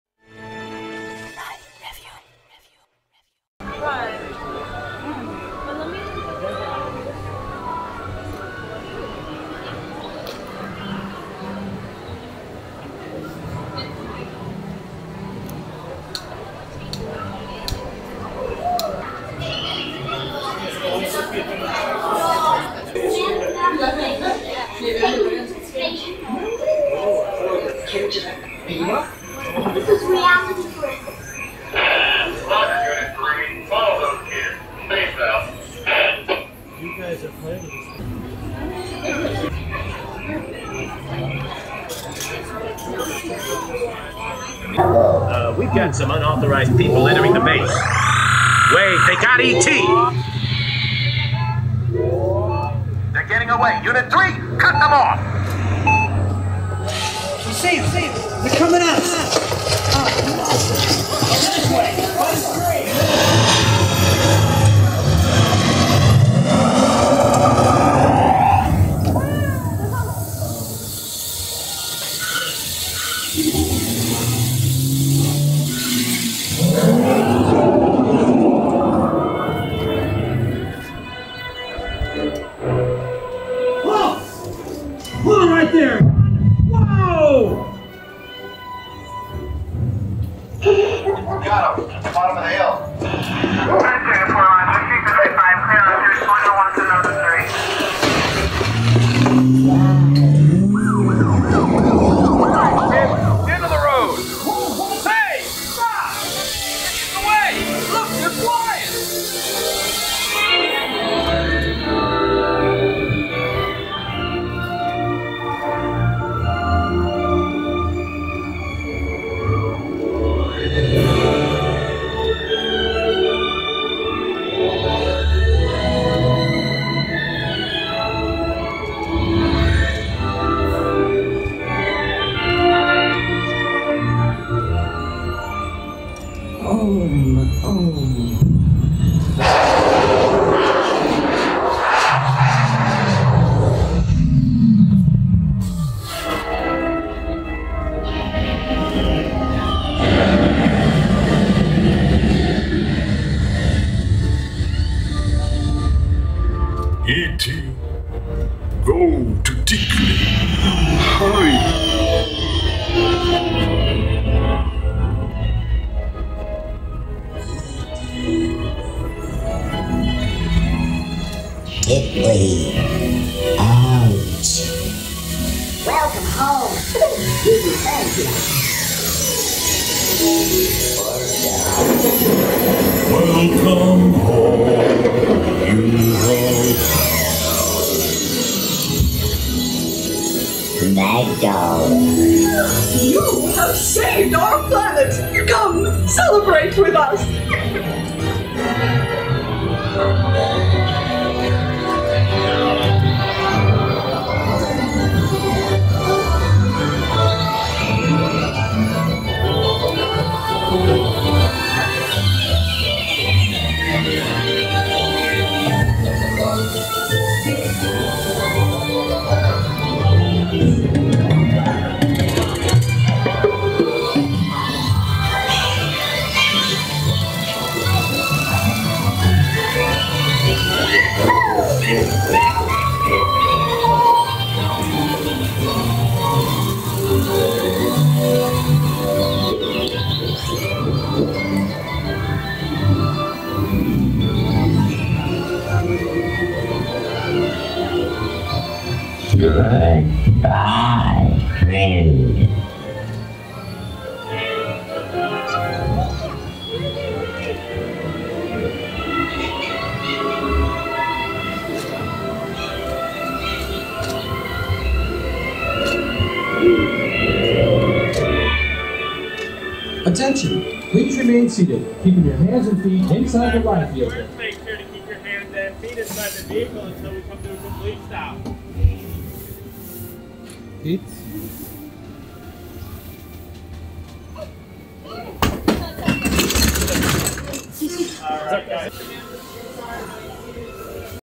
E.T. Adventure - POV - Universal Studios Orlando - Sally Corporation / Birket - Suspended Dark Ride
E.T. Adventure - POV - Universal Studios Orlando - Sally Corporation / Birket Engineering / Universal Creative - Suspended Dark Ride

E.T. the Extra-Terrestrial